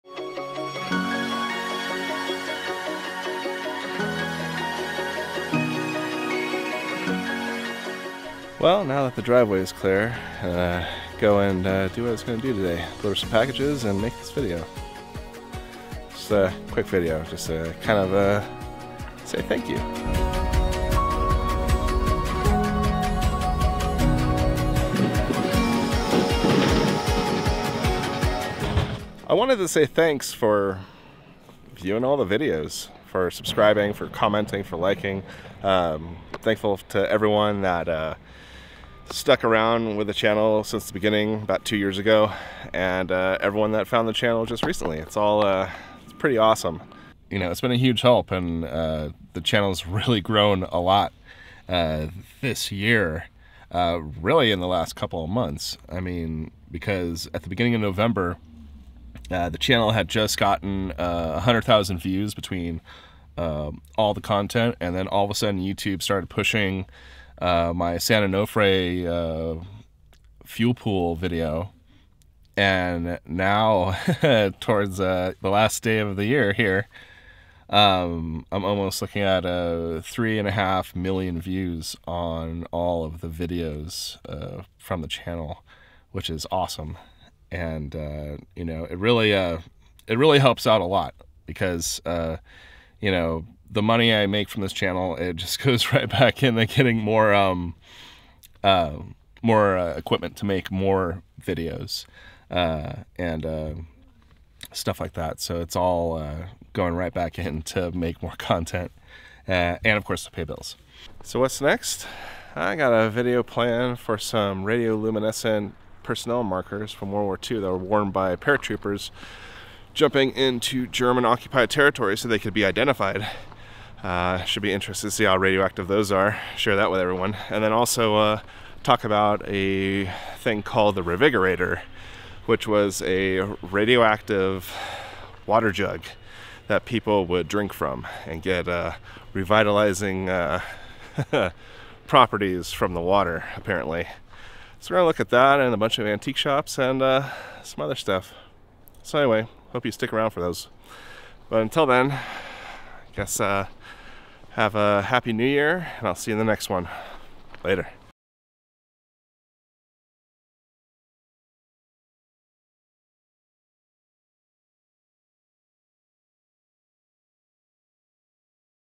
End of the Year Thanks for over 3 Million Views in 2 Months
Saying thanks to everyone that has helped this channel grow so much this year. In November this channel started getting promoted by YouTube, which was a huge help in its growth. But everyone liking and commenting on all the videos helped so much as well. I hope everyone has a great New Year.